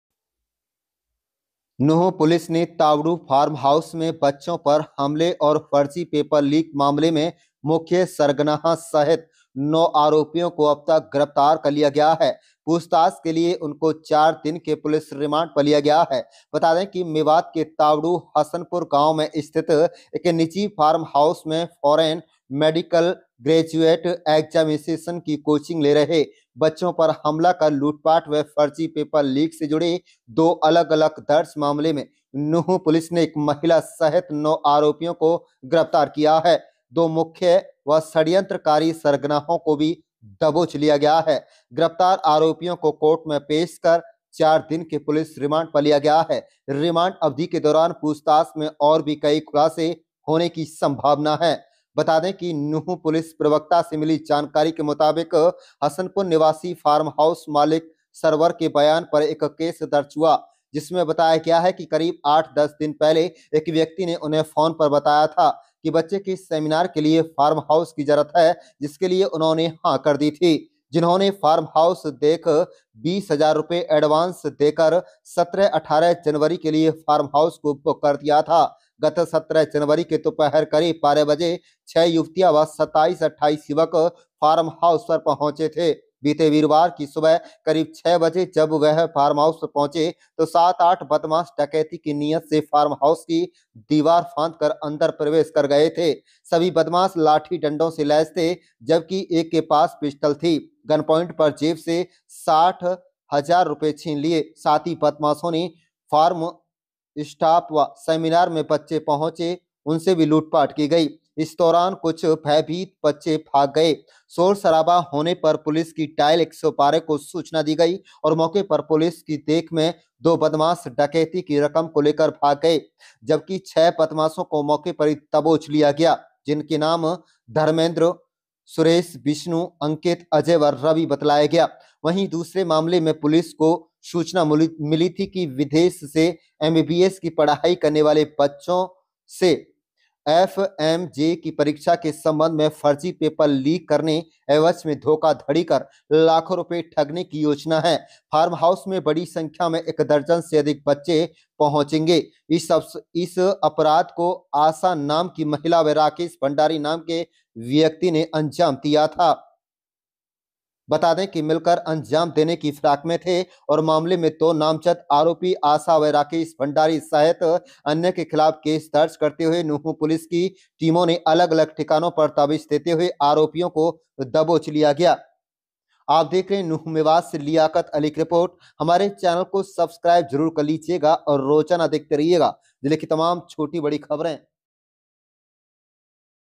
तावडू फर्जी पेपर लीक मामलें में मुख्य सरगना सहित 9 गिरफ्तार ● पूछताछ के लिए चार दिन पुलिस रिमांड पर
tavdu_sohna_(Mewat)_Leader

mewativideo_rajankhan_hakkusi ngarya_mewat